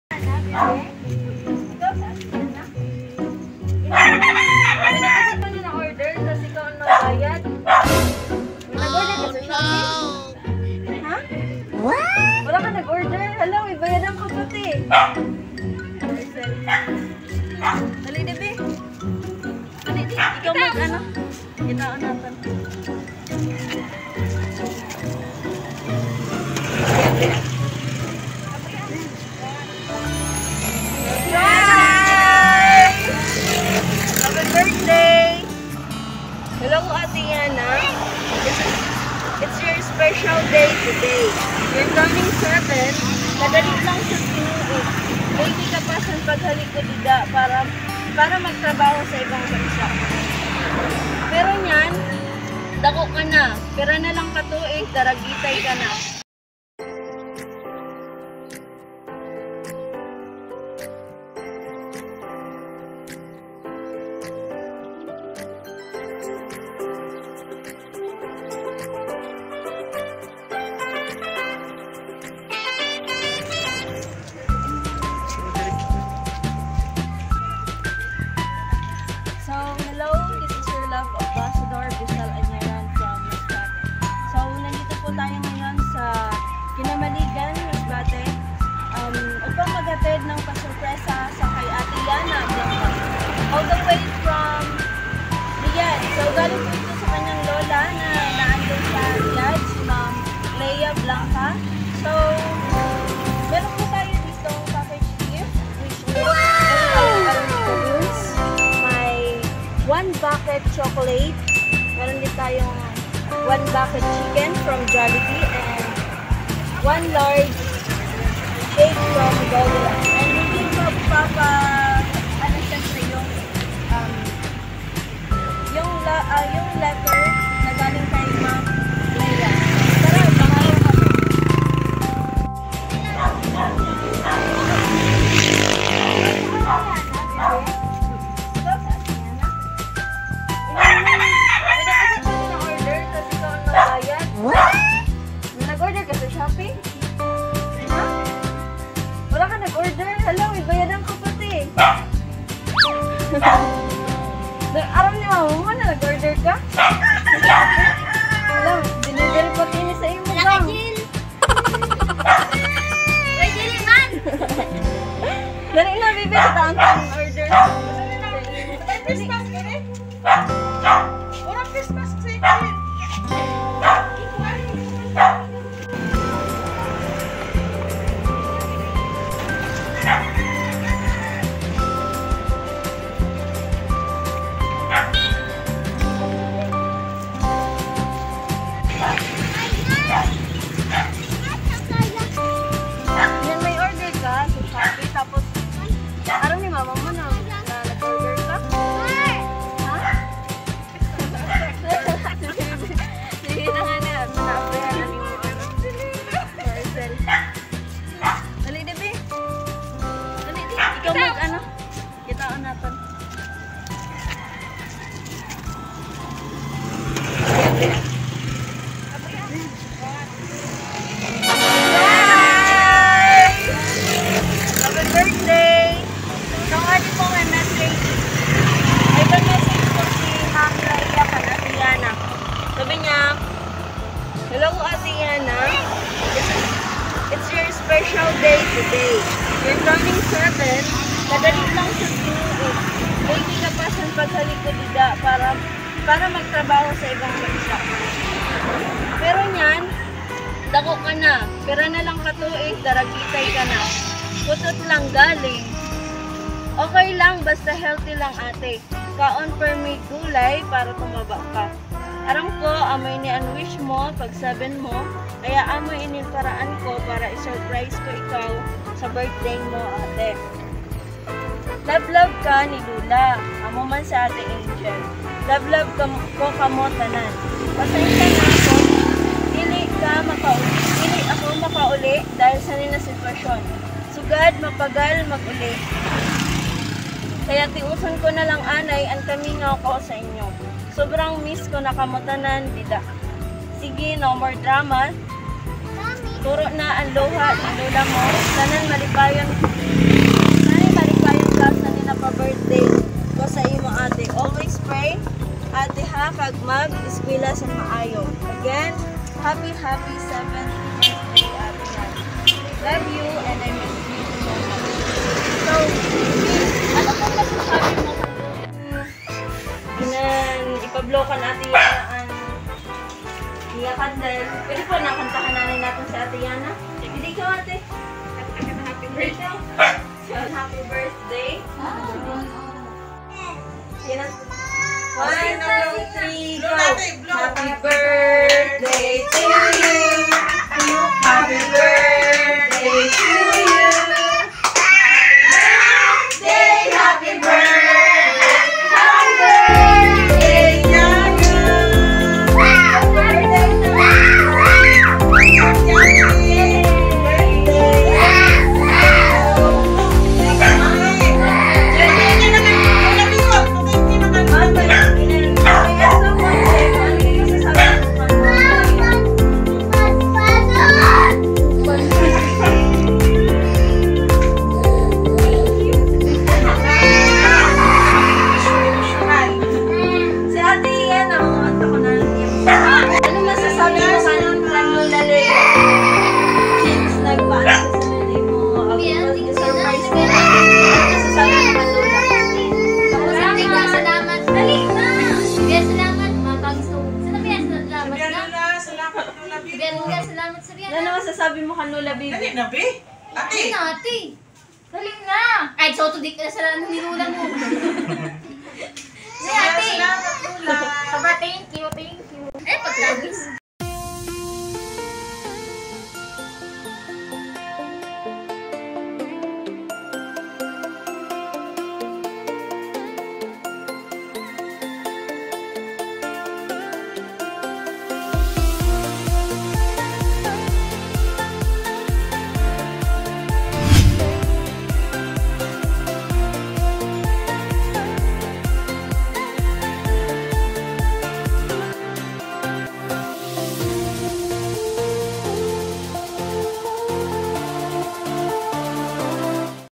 LOLA WILL ALWAYS LOVE YOU APO! ❤😍
Napasaya naman talaga ni Lola Leah ang kanyang mga apo sa kanilang Birthday, Tuwang tuwa sila sa mga gifts and surprise!. Halina at panoorin natin ang reaksyon ng mga Apo ni Lola Leah Dito lang sa SendLove2pinas!!. ☺💕💕❤😍😍❤

Sending Love from Saudi Arabia 🇸🇦 to Pinas 🇵🇭

To: YHANNA BLANCA
From: LEAH BLANCA

Sendlove2pinas delivered FoodTreat as follows

Arranged Balloons
LARGE Red Ribbon Cake
1bucket of Mixed Chocolate worth 2K
Jollibee / KFC Bucket

SPECIAL MESSAGE:

Hello Ate yhanna ❤️it's your special day today, your turning 7 kadali lng sn mga tinuig baby ka pa sn pghali ko dida para mgtrabho sa ibang bansa ❤️pero nyan dako ka na pira nlng katuig daragitay ka na putot lng galing 🤣OK lng basta healthy lng ate kaon permi gulay para Tumaba ka 🤣Aram ko amo inin wish mo pag 7 mo kya amo inin paraan para I surprise ko ikaw sa birthday mo ate love love ka ni Lola amo mn c angel love love ko kmo tanan ❤️pasinsya na qn dli ako mkauli dahil sani na setwasyun sgd mapagal mag uli kya tiuson q nlng anay an kamingaw q sa inyu, subrang miss ko na kmo tanan dida 😪😪😪Cge na no more drama na turo na luha ni Lola mo, tani malipayon ka sani na pabirthday q sa imo Ate, always pray ate ha kag mag eskwela sn maayu, again HAPPY HAPPY 7TH BIRTHDAY ATE YHANNA ❤️LOVE YOU AND MISS YOU ALL SO MUCH ❤️THANKYOU SO MUCH DIN PO SA EFFORT NG TEAM SENDLOVE2PINAS MARAMING SALAMAT SA INYU GODBLESS YOU ALL 🙏❤

HOW?!

Thanks!
Spread Love! 💕